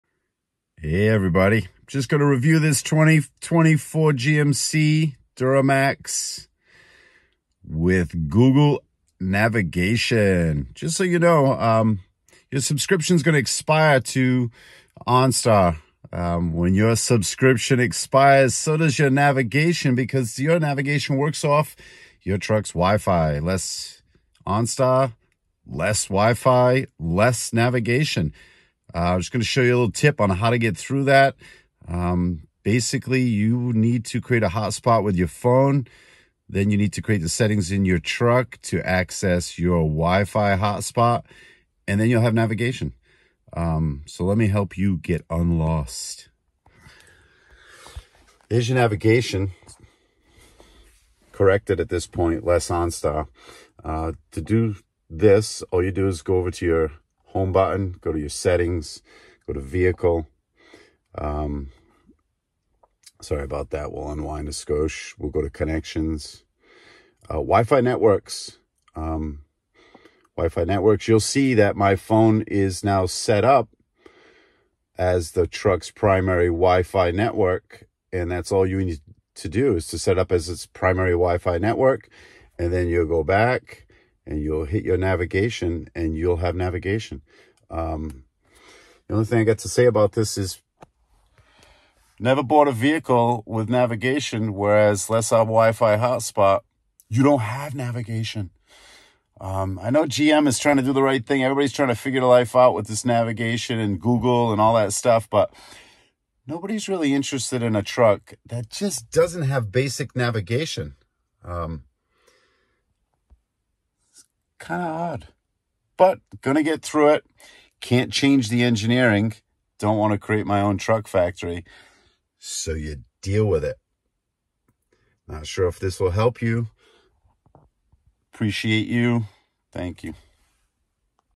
2024 GMC duramax navigation lost without onstar and Wi-Fi subscription
2024 GMC duramax Lost with out onstar